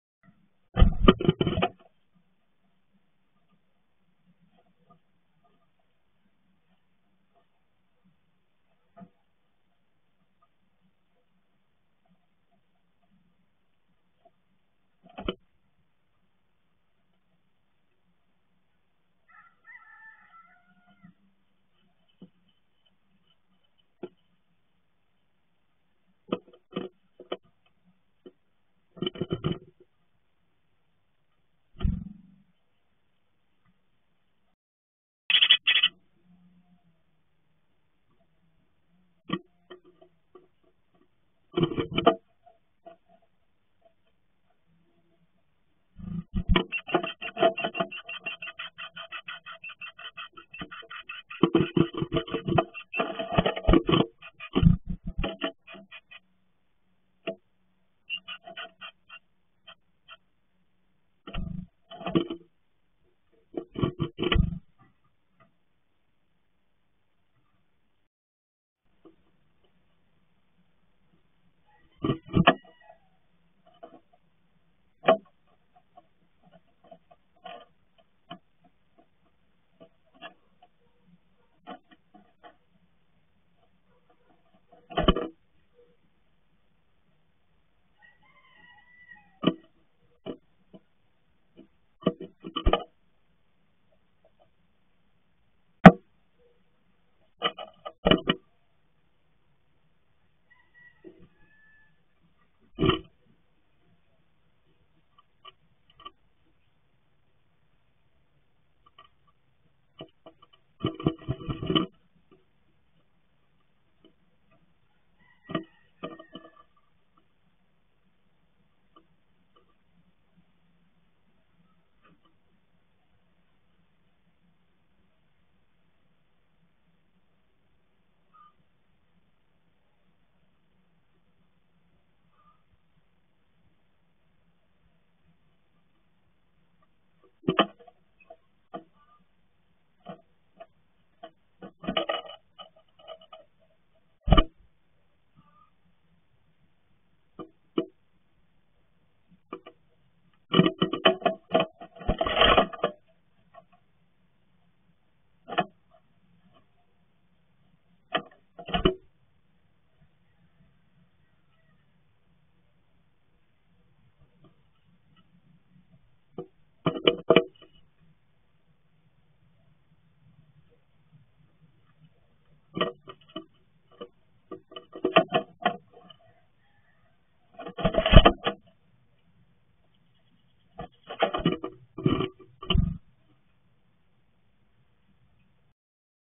Trei zile cu vizite până pe 21 octombrie 2024
Trei zile cu vizite până pe 21 octombrie 2024: 6,8 li 21 octombtie.Pe data de 15 otombrie a fost mare agitațe afară și la un moment dat erau doi pițigoi mari în interior